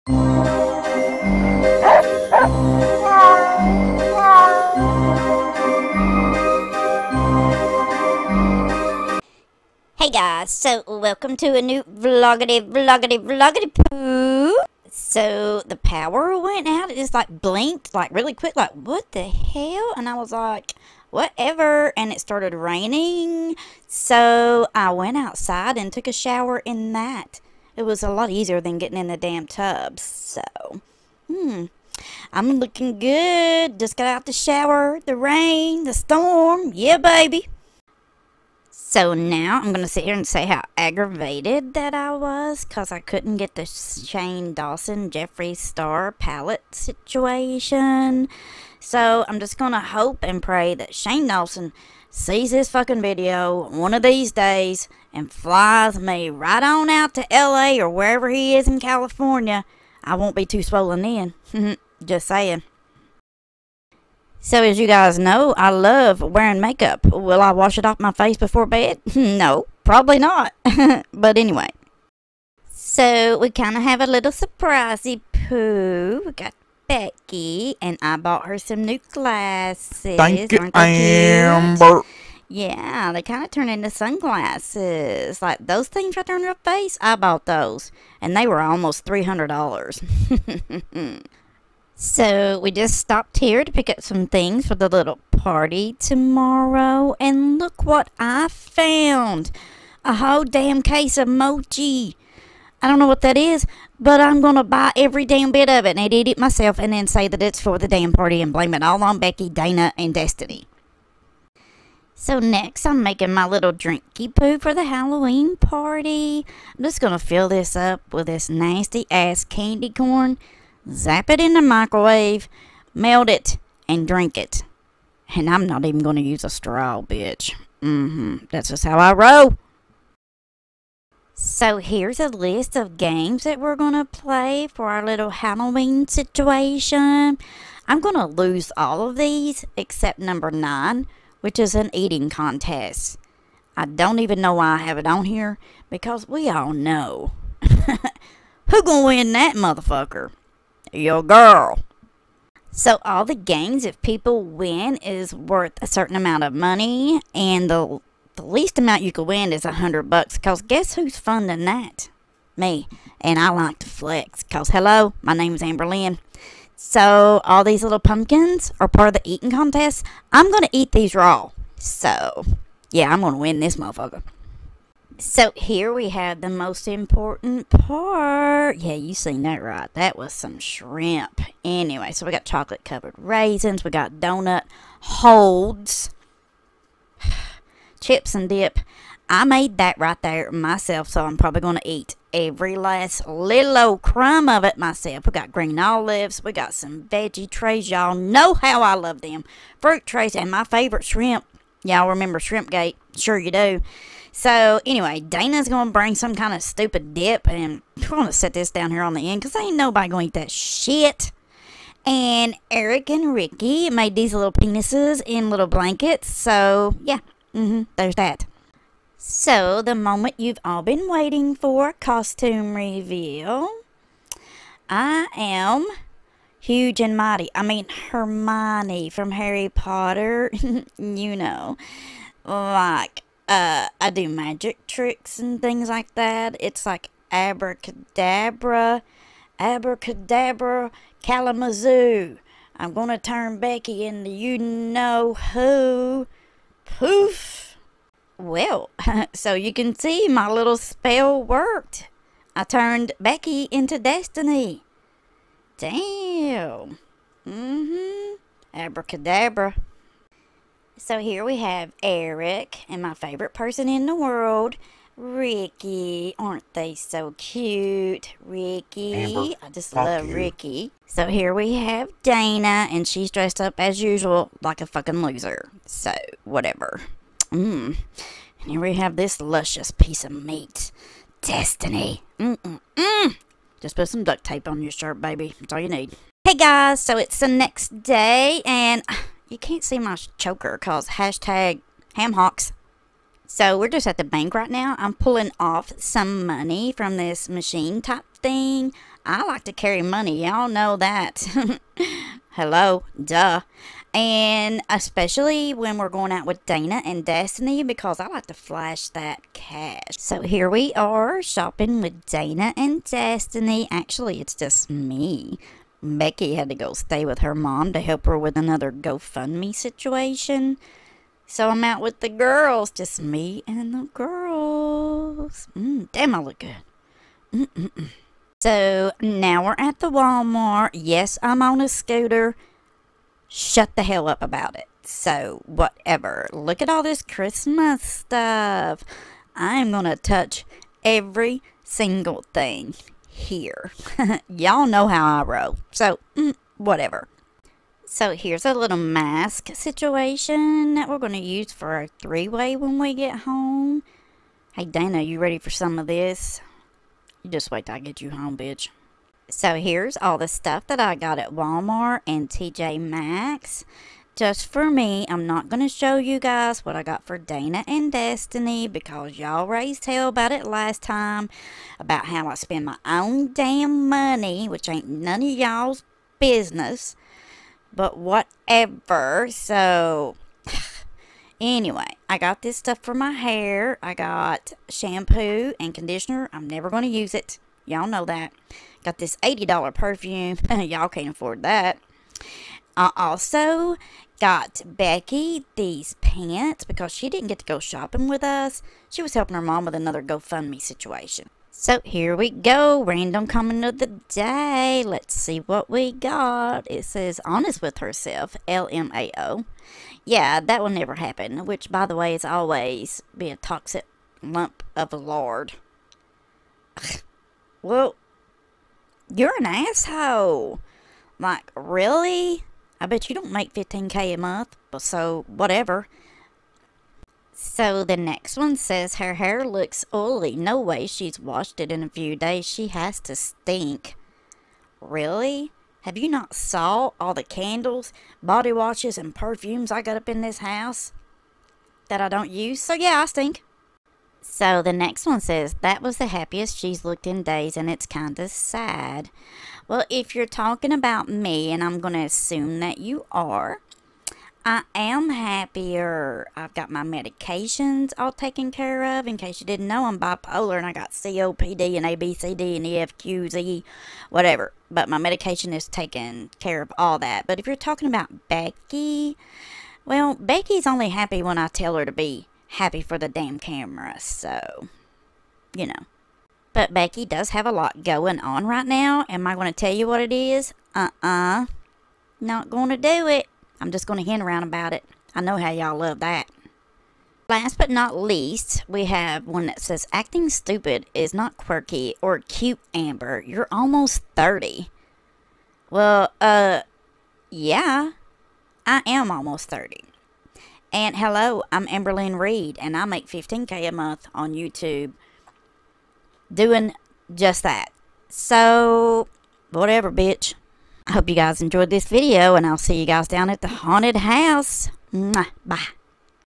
AmberlynnReid/ALR/Halloween Special/The REAL ALR
What Amberlynn REALLY wanted to say in her halloween video!